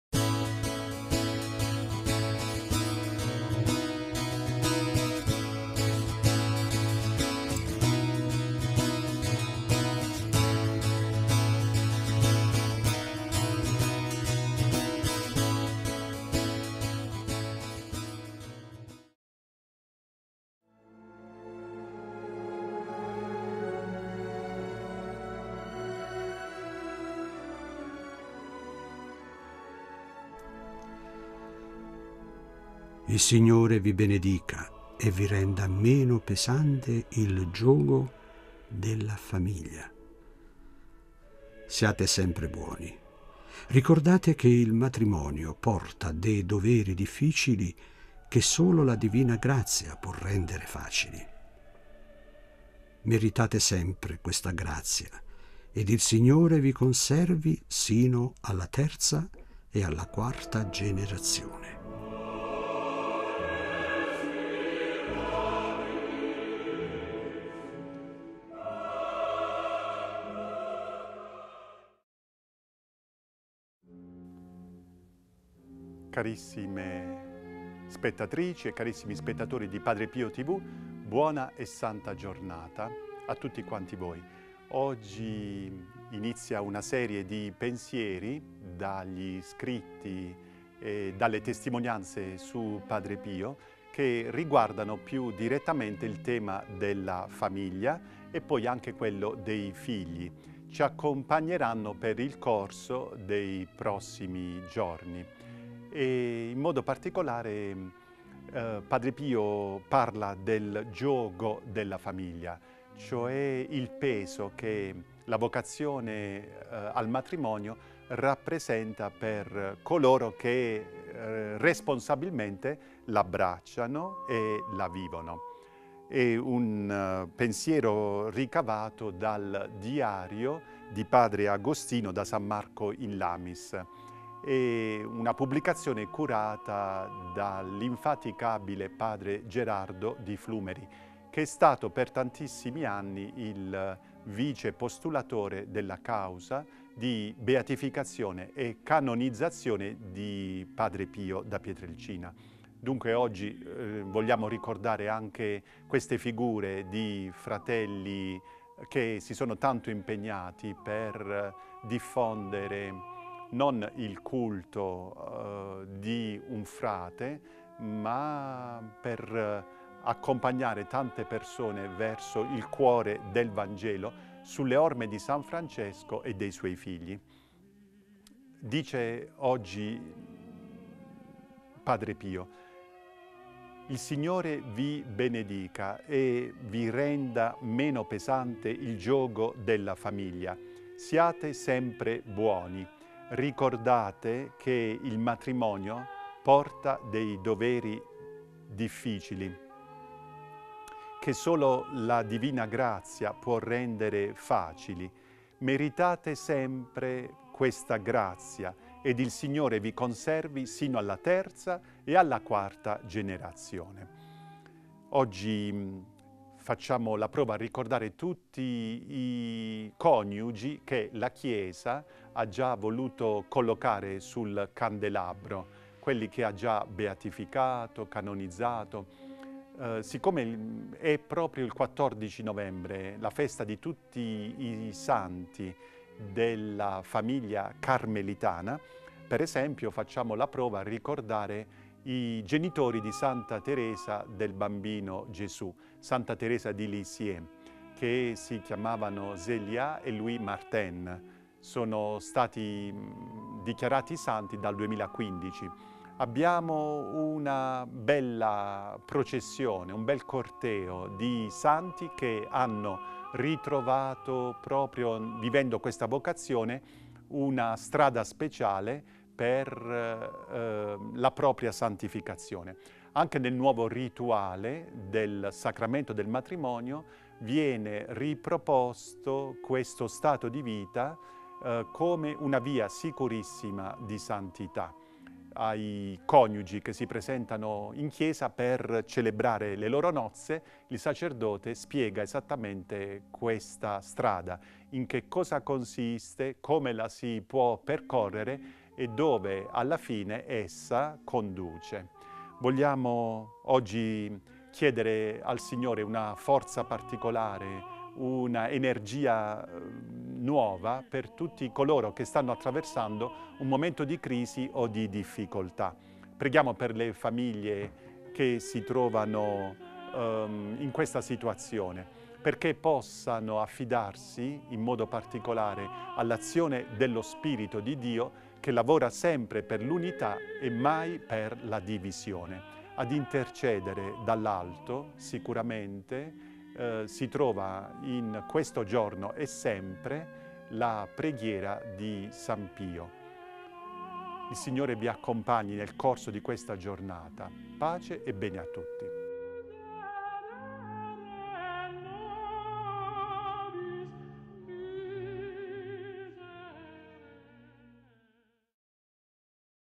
Chi si attacca alla terra, ad essa resta attaccato
Fra PierGiorgio Taneburgo - Buona Giornata con Padre Pio 14 Novembre 2019
Ogni giorno i Frati Cappuccini augurano la buona giornata con le parole di Padre Pio alle ore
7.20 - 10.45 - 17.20
La Frase di Padre Pio:
"Chi si attacca alla terra, ad essa resta attaccato. È meglio staccarsi poco per volta, anziché tutto una volta. Pensiamo sempre al cielo"